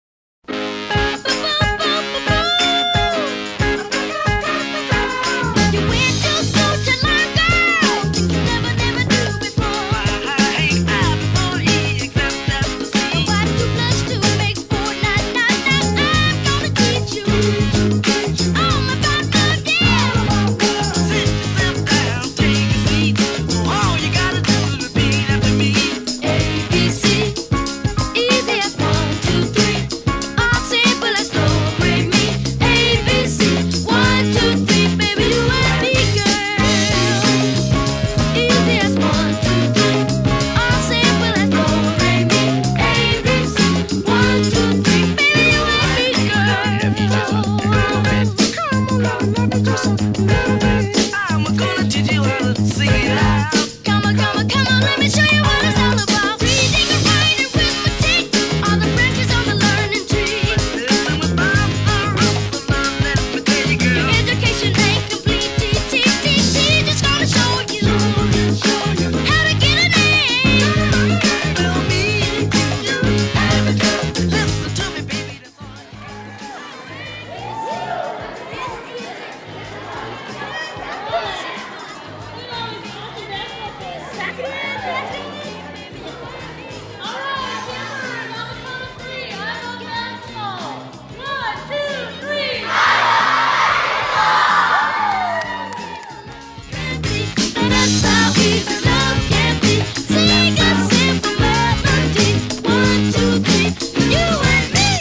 Jr Tiger Clinic.m4v
We held our first of five Jr Tiger clinics for the season today! 110 kids between the ages of kindergarten to 8th grade participated. We love teaching the game of basketball!